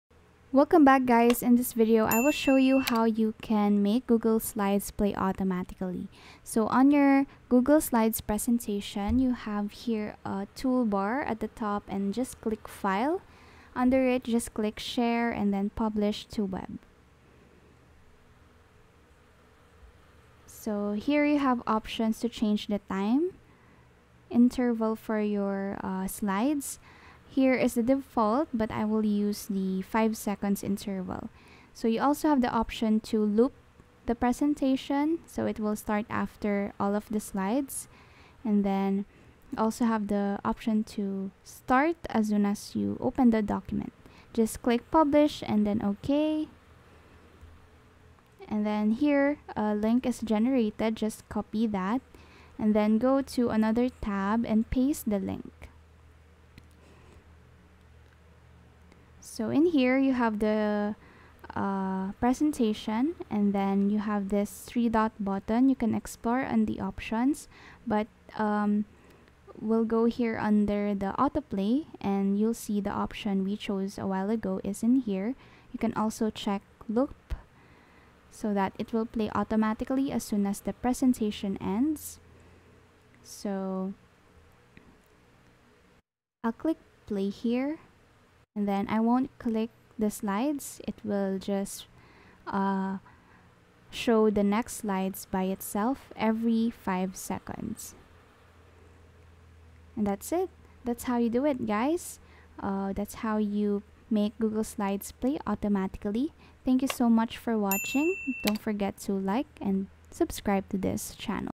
How To Make Google Slides Play Automatically (EASY!)
How To Make Google Slides Play Automatically . It’s a very easy tutorial, I will explain everything to you step by step.